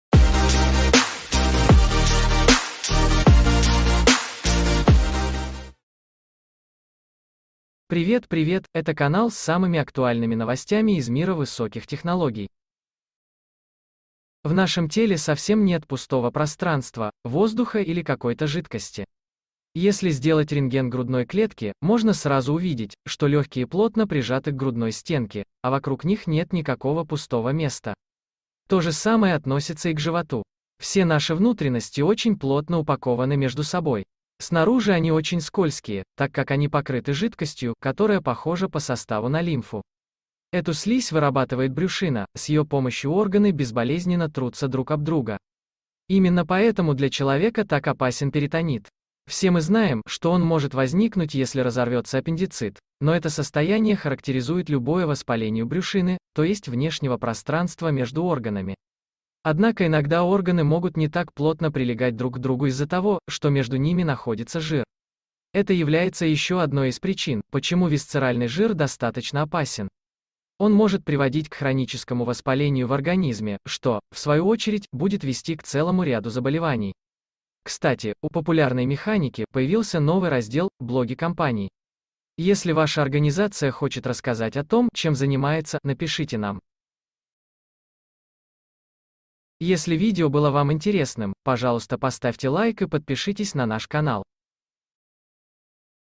Что находится между органами
Когда жидкость в нашем животе начинает урчать или забавно хлюпать, кажется, что все органы внутри просто “плавают” в пустом пространстве. Но это совсем не так! Благодаря компьютерной томографии, МРТ и рентгену специалисты уже давно знают, что же находится между органами, спойлер: ничего!